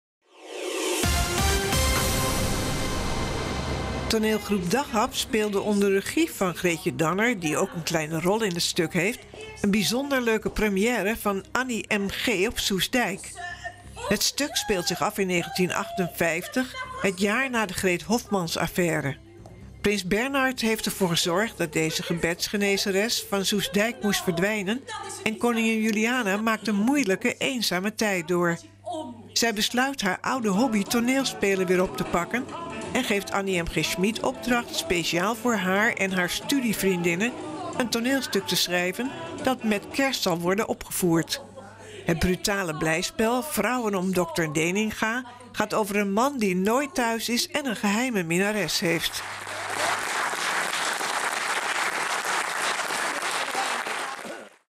D@gh@p speelt 'Annie MG op Soestdijk'
Toneelgroep D@gh@p speelde zaterdag de première van 'Annie MG op Soestdijk'.

Een video van Kitty Mertz en Ed van Oerle.